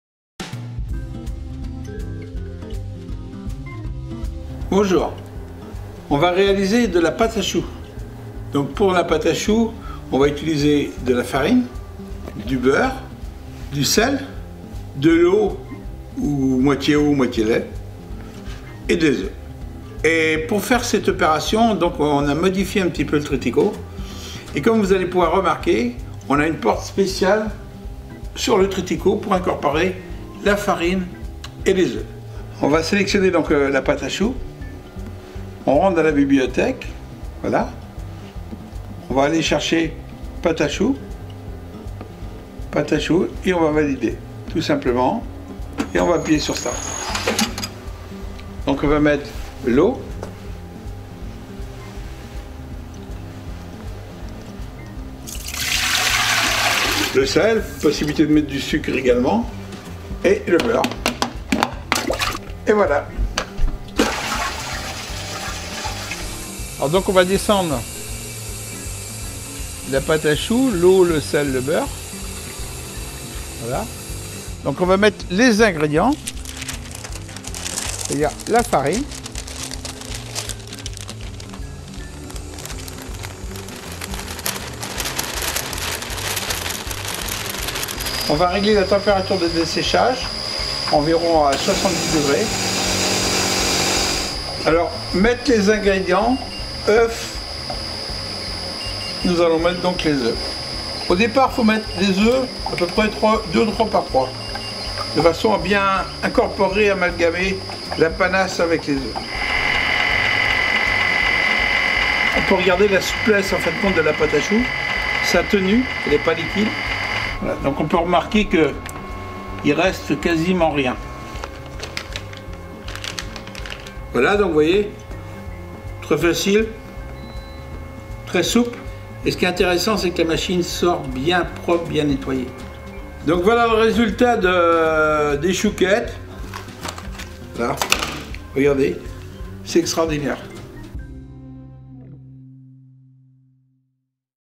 Pate a choux with Trittico Executive Evo (EN)
This video shows how to make PATE A CHOUX with Trittico Executive Evo, the Bravo pastry machine that simplify and automate the work of a pastry chef and ensures constant quality to his products!

Remember to check the availability of this program for the software of Trittico you want to purchase.To make PATE A CHOUX with Trittico executive evo you must buy the PATE A CHOUX kit (configurator.bravo.it). Ask for more information to